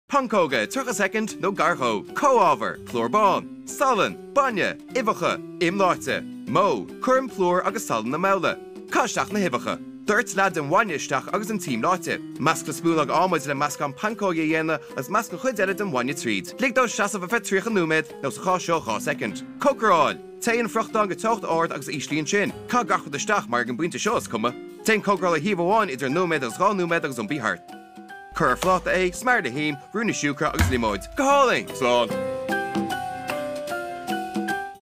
Pancóga i 30 soicind (nó gar dhó) | Pancakes in 30 seconds | BLOC
Caith súil ar oideas pancóige Bloc!

Have a look at Bloc’s pancake recipe!

Is féidir teacht leathanaigh BLOC anseo ➡️